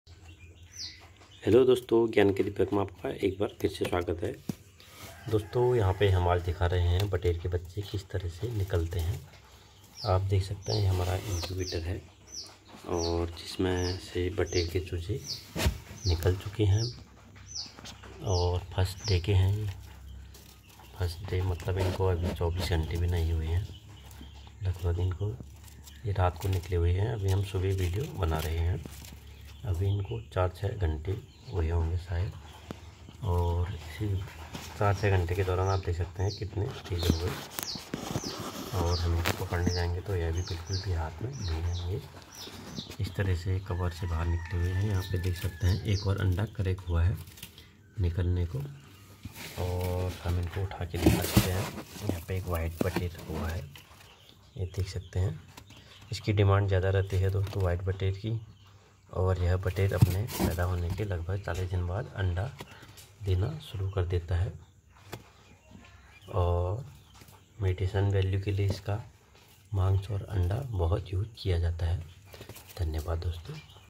Live Egg Hatching Video // Egg Incubator Machine // Homemade Incubator Information/ Results
Egg Hatching Video In Hindi